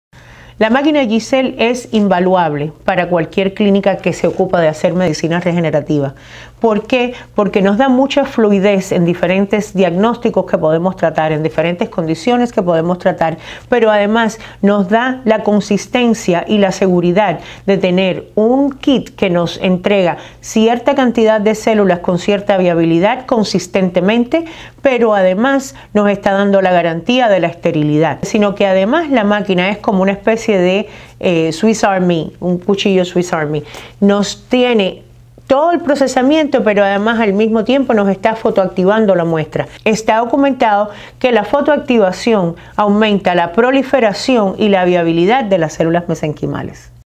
Fotoactivación en Muestras | Medicina Regenerativa
Gcell nos da fluidez para trabajar en la Medicina Regenerativa. Además de la Seguridad, Esterilidad, Conteo y Viabilidad de la muestra. También fotoactiva la muestra para un tratamiento más optimo.